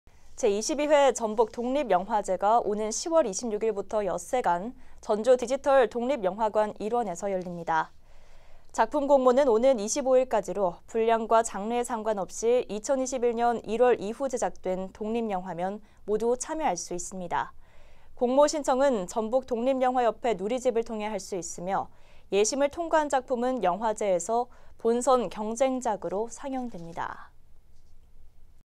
제22회 전북독립영화제 10월 개최…출품작 공모
제22회 전북독립영화제가
오는 10월 26일부터 엿새 간
전주디지털독립영화관 일원에서
열립니다.

작품 공모는 오는 25일까지로,
분량과 장르에 상관 없이
2021년 1월 이후 제작된 독립영화면
모두 참여할 수 있습니다.

공모 신청은
전북독립영화협회 누리집을 통해
할 수 있으며, 예심을 통과한 작품은
영화제에서 본선 경쟁작으로
상영됩니다.